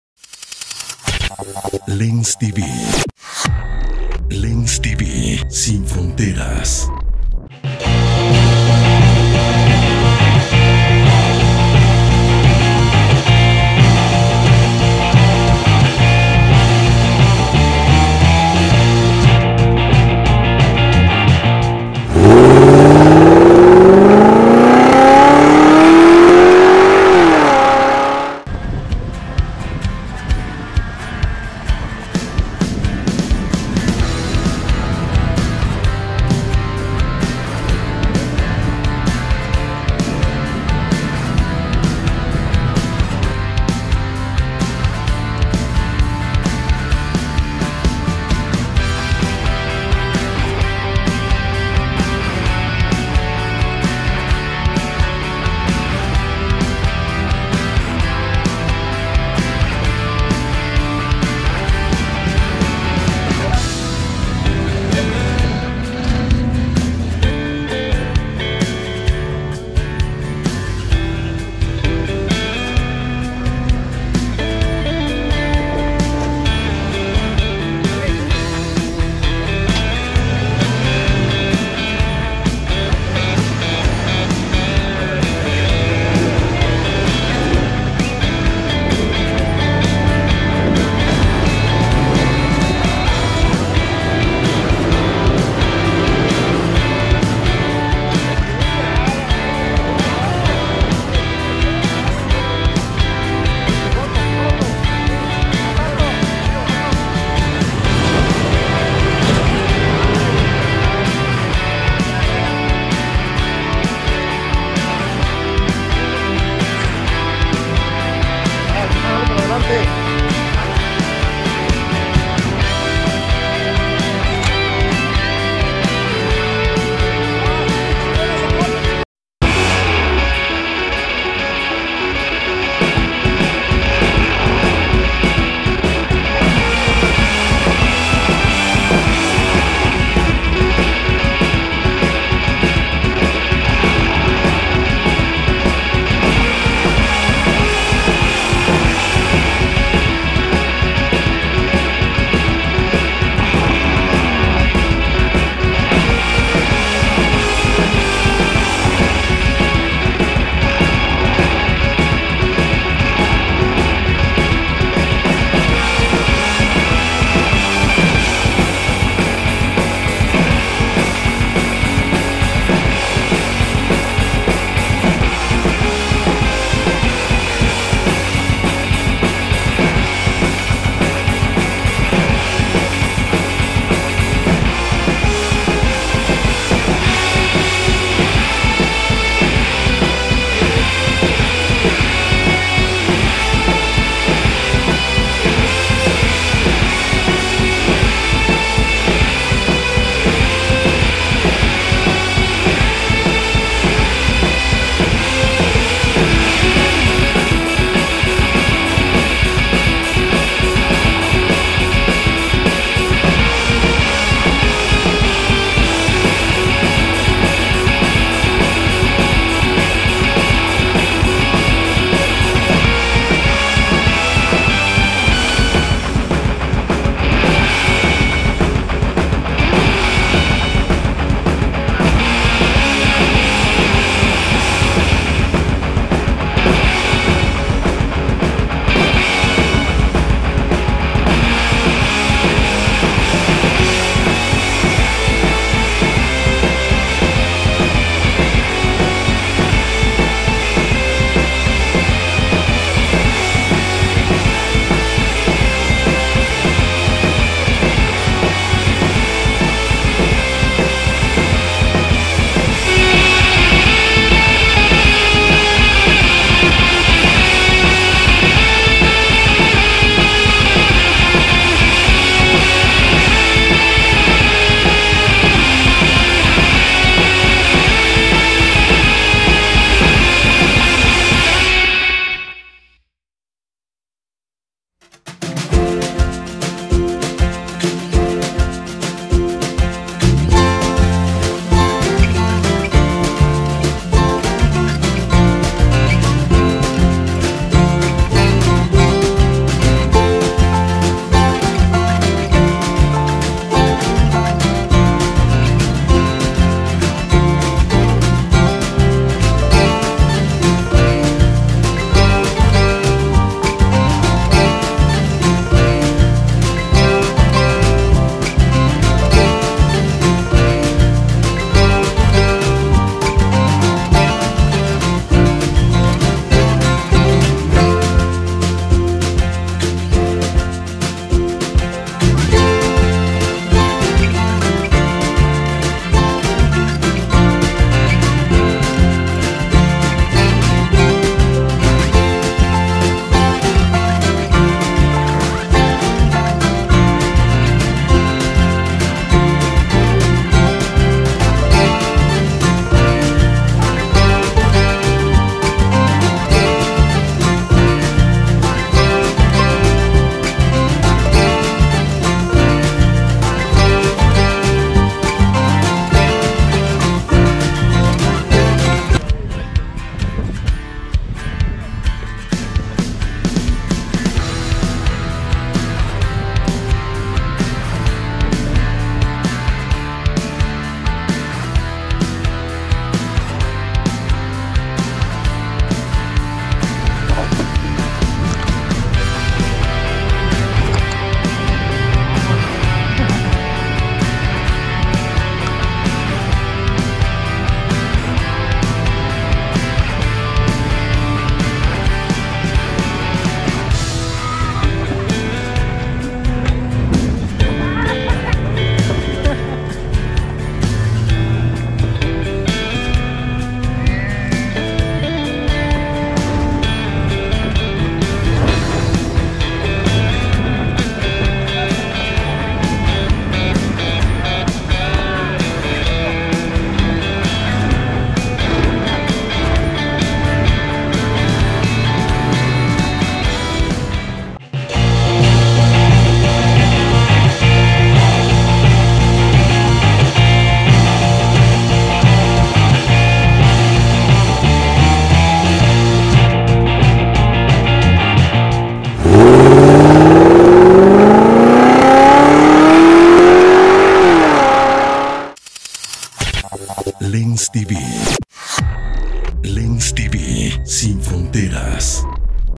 ⭕ LensTV Ruta Galerías 4X4 ADVENTURE ZACATECAS Agosto 2022
⭕LensTV Ruta Galerías 4X4 ADVENTURE ZACATECAS
Ahora em Turbo cargado te compartimos unos pasos en vehiculos todo terreno en la capital zacatecana. A los amantes de este deporte extremo chequen esos movimientos y más.